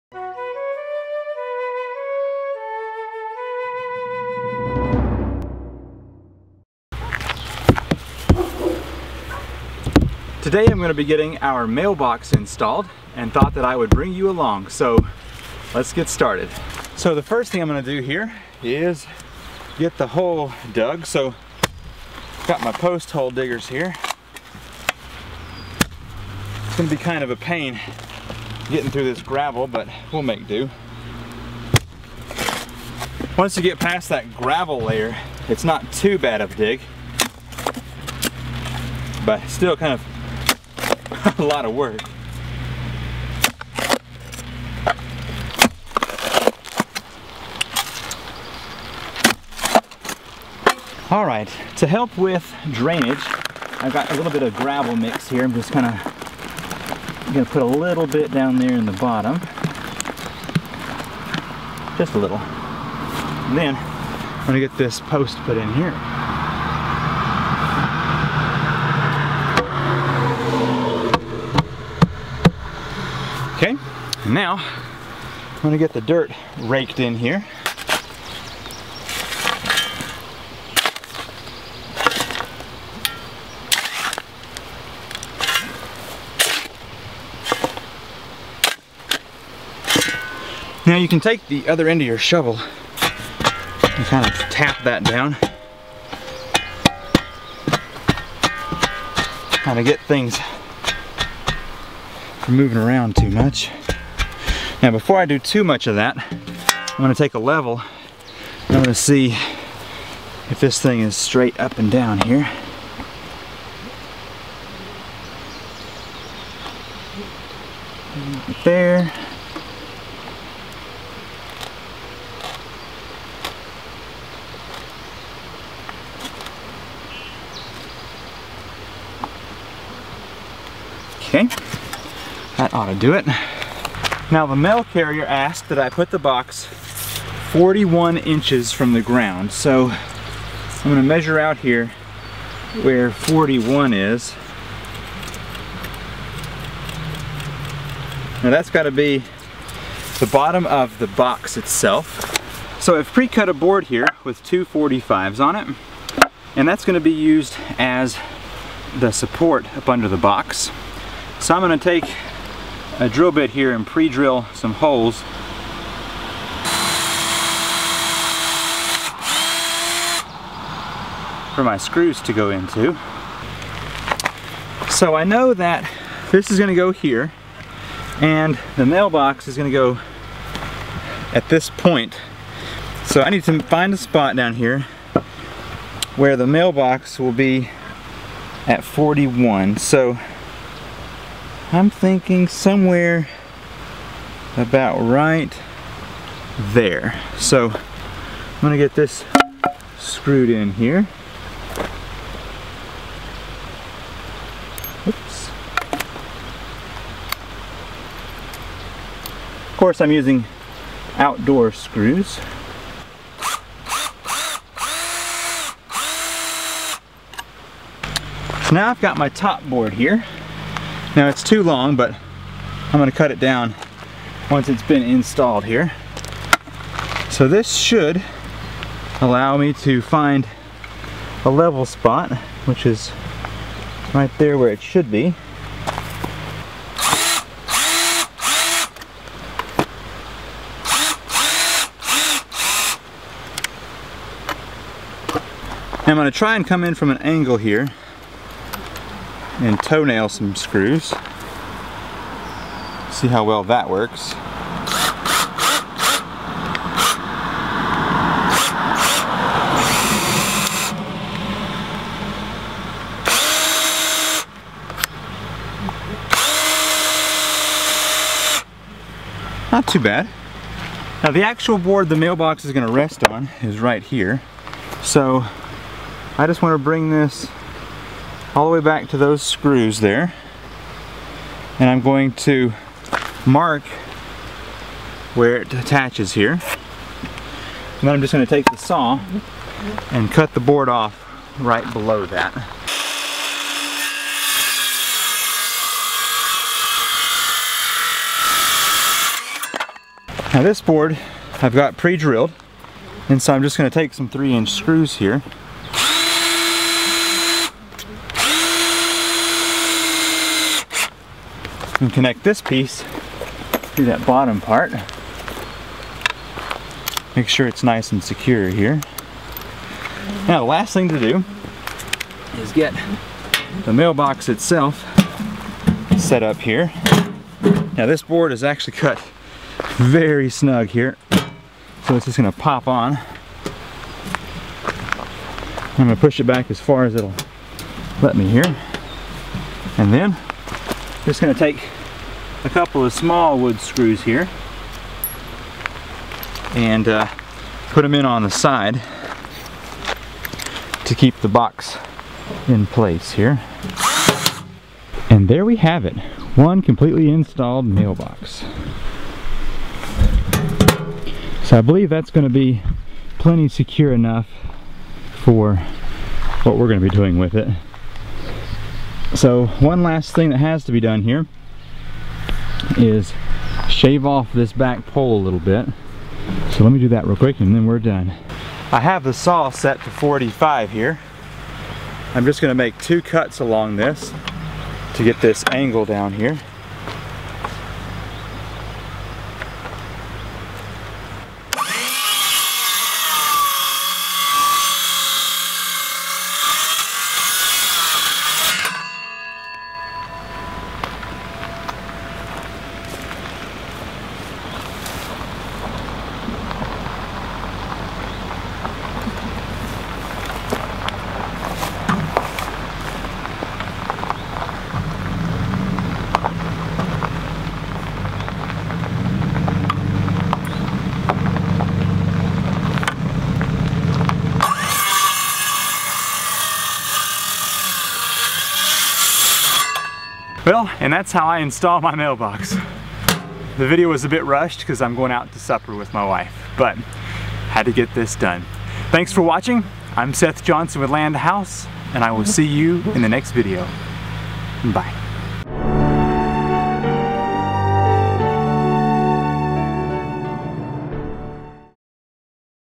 Install the Mailbox
Now that we have a new house its time to get the mailbox installed. I know there are many nice posts you can buy or designs that you can build but in this video I am going to install a basic mailbox on a simple frame.